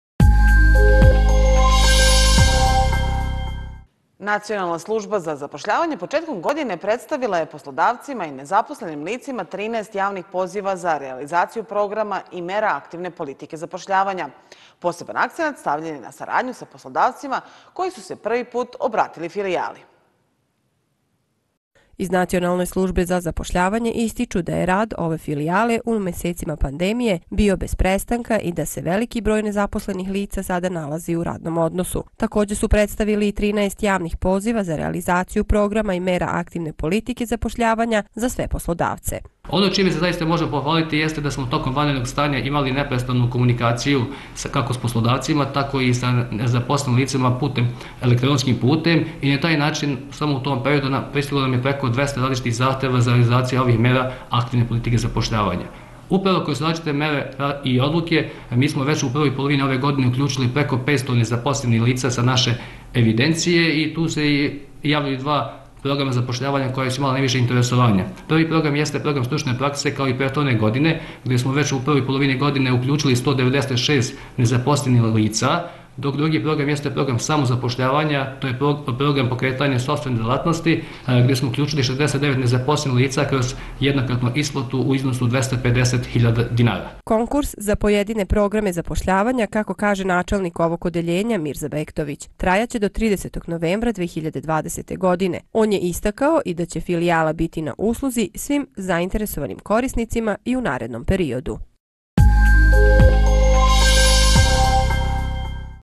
NSZ: Programi i mere zapošljavanja daju rezultate
Nacionalna služba za zapošljavanje početkom godine predstavila je poslodavcima i nezaposlenim licima 13 javnih poziva za realizaciju programa i mera aktivne politike zapošljavanja. Poseban akcenat  stavljen je na saradnju sa poslodovacima koji su se prvi put obratili Filijali.